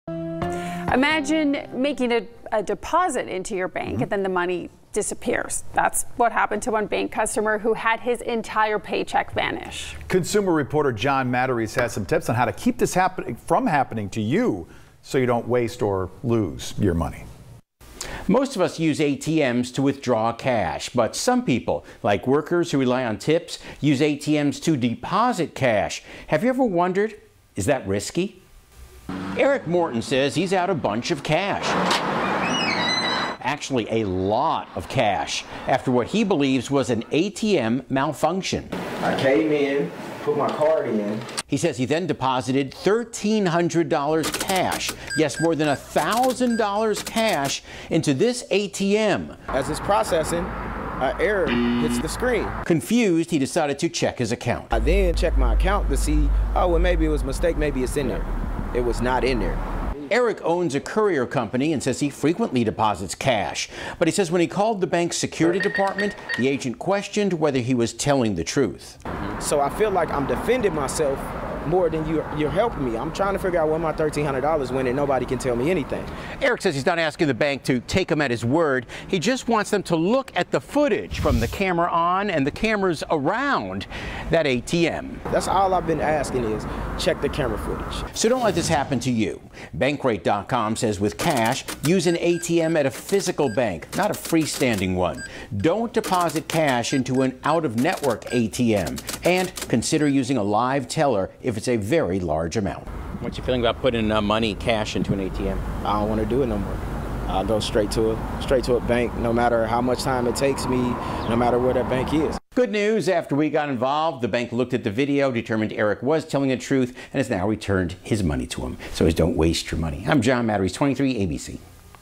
How safe is it to deposit cash in to an ATM?
Imagine making a deposit into your bank and then the money disappears. That's what happened to one bank customer who had his whole payday vanish.